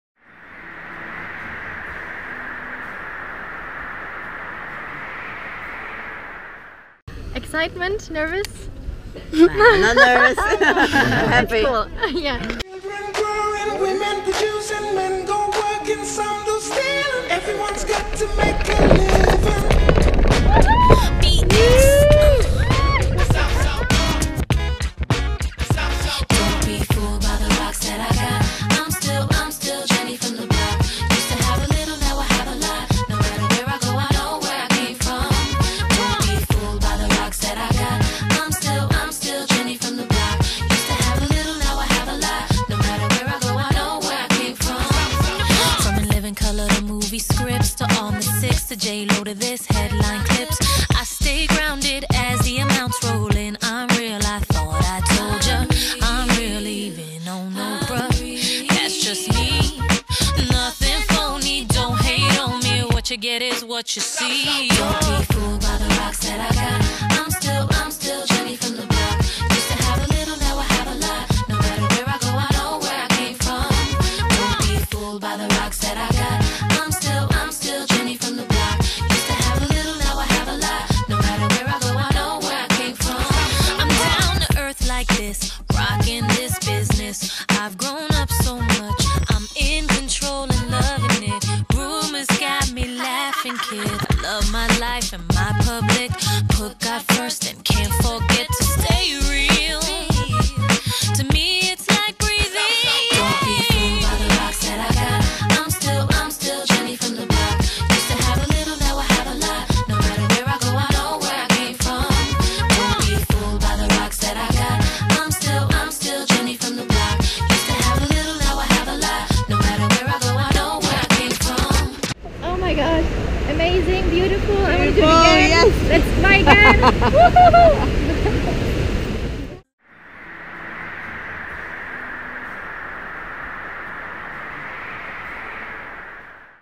Hanggliding in Rio de Janeiro with ANNIE
Come to Rio and hang glide with Rio Asa Delta!
Amazing experience.